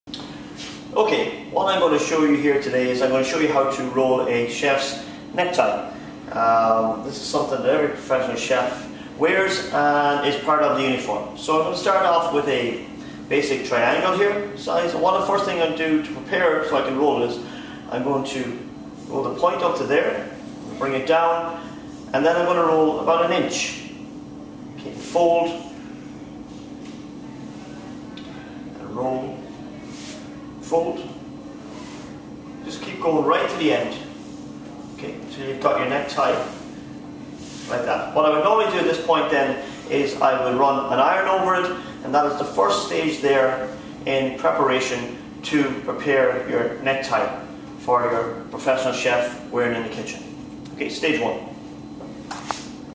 Prepare a Chef Necktie
Basic instruction on the preparation method for rolling a chef Necktie